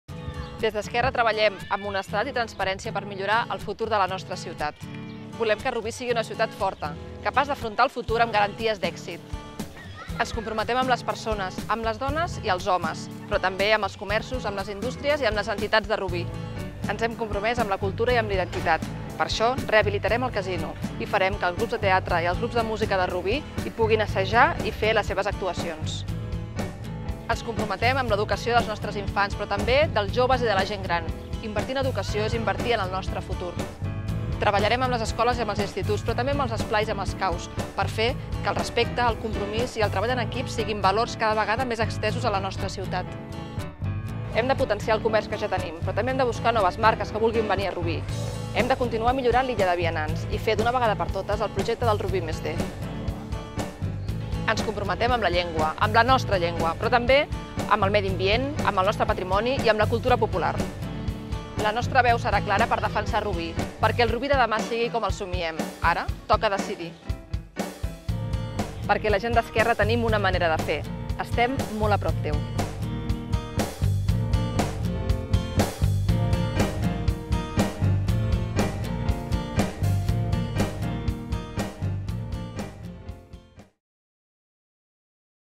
Estem molt a prop teu (IV)
Vídeo de campanya d'Arés Tubau per a les Eleccions Municipals de Rubí del 2011. Candidata alcaldable per Esquerra a Rubí
Vídeo de campaña d'Arés Tubau para las Elecciones Municipales de Rubí en 2011.

A Esquerra Rubí estem molt a prop teu!